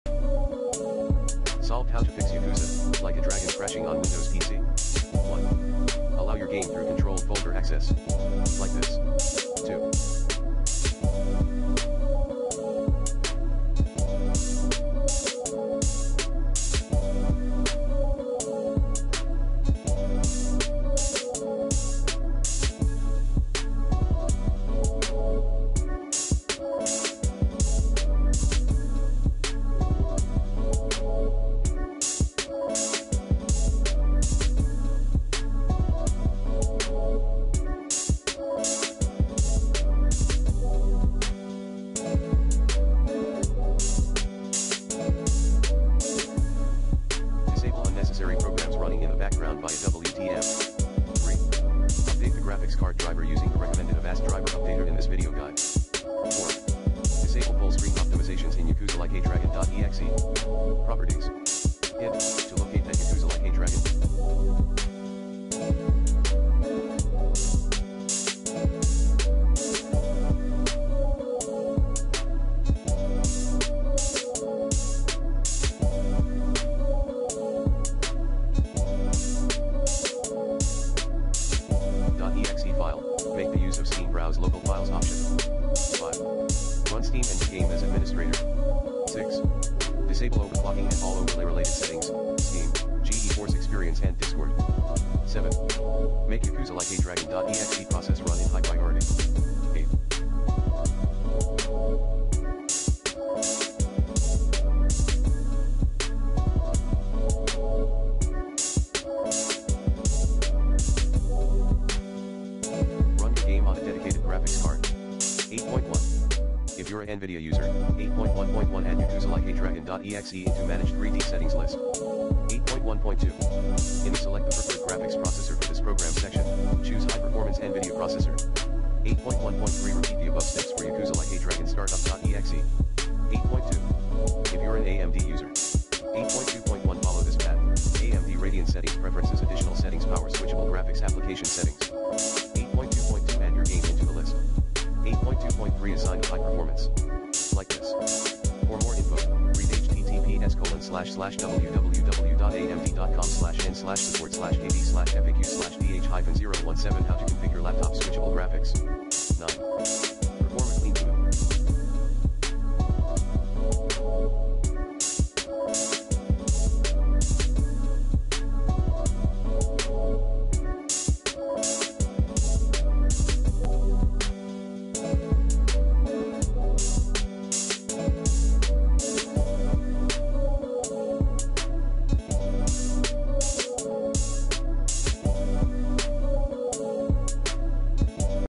Fix Yakuza: Like a Dragon Crashing on Windows PC
Here's how to Fix Yakuza: Like a Dragon Crashing on Windows PC. Run Avast Driver Updater@ to keep your device drivers up-to-date, easily & effectively.